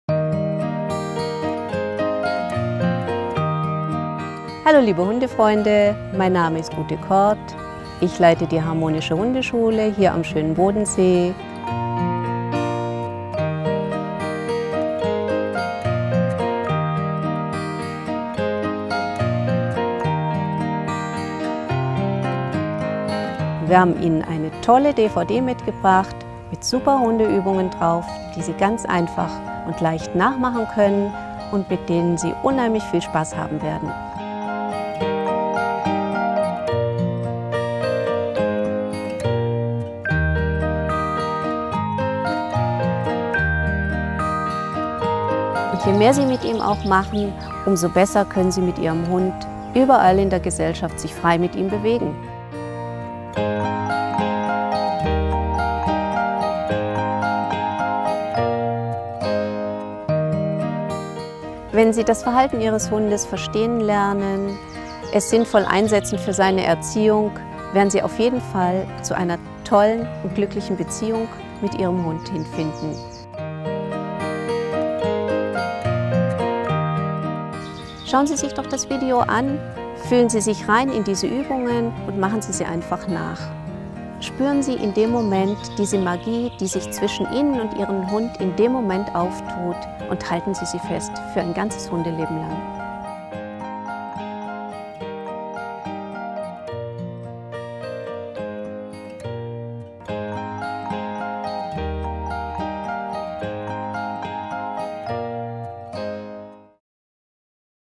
Harmonisches Hundetraining mit Hundetrainerin Ute Kordt
Basisübungen & Tricks für Hunde vom Welpenalter bis zum Seniorenalter, die Spaß, Lebenfreude und eine vertrauenvolle Bindung zwischen Dir und Deinem Hund schaffen.

Erfolgreiches Hundetraining basiert auf Aufmerksamkeit, Zuwendung und einem harmonischen Miteinander.

Im Gegensatz zu vielen anderen Hundetraining-DVD´s  zeigt Dir die Hundetrainerin Ute Kordt auf der DVD Harmonisches Hundetraining:

einfach anwendbare Übungen für den Alltag und die Freizeit
Basisübungen & Tricks
in detaillierter Anleitung dirket mit dem Hund

Also keine theoretische DVD sondern praktisch gezeigte Übungen mit vielen Hinweisen und Tipps was im Hundetraining und den einzelnen Übungen wichtig ist.

Dabei steht die Harmonie und das Vertrauen zwischen Dir und Deinem Hund immer im Vordergrund.

Zusätzlich zur DVD sind alle Übungen und Tricks auf dem beiliegenden Trainingsposter nochmal in Stichpunkten gezeigt.